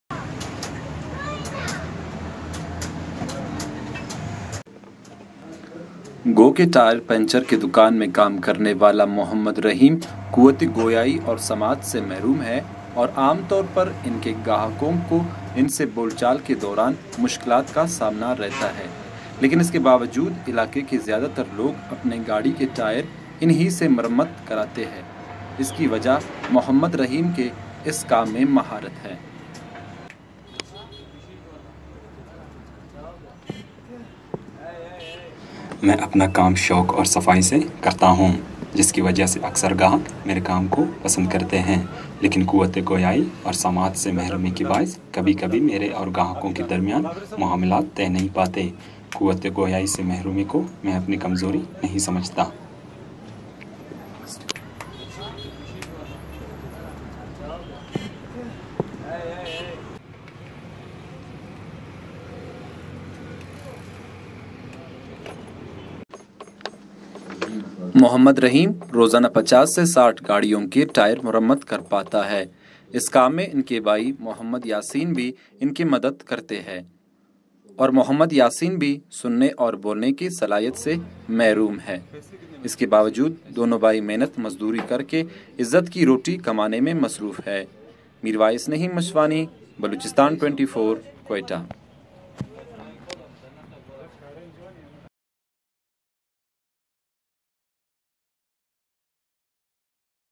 STORY OF 23 YEARS HOLD BOY
a short story of person who is not having ability of speaking and listonning  but inspit of that working in tire reapiring shop in QUETTA